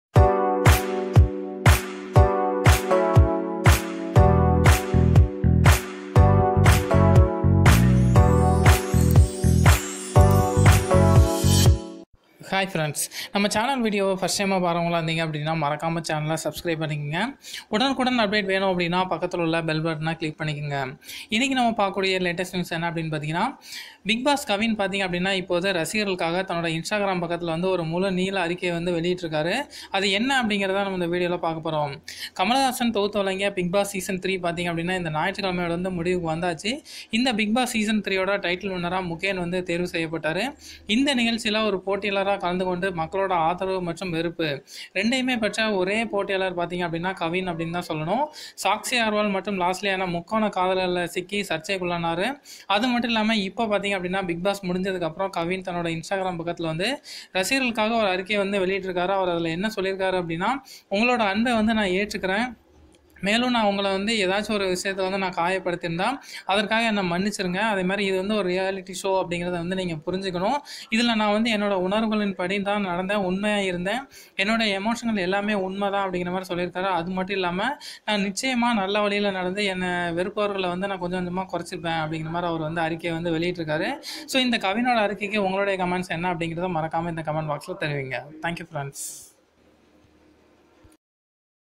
என்னை மன்னிச்சிடுங்க - Kavin Post After Bigg Boss to Fans | Kavin Army | Losliya
Videos From Muthuravi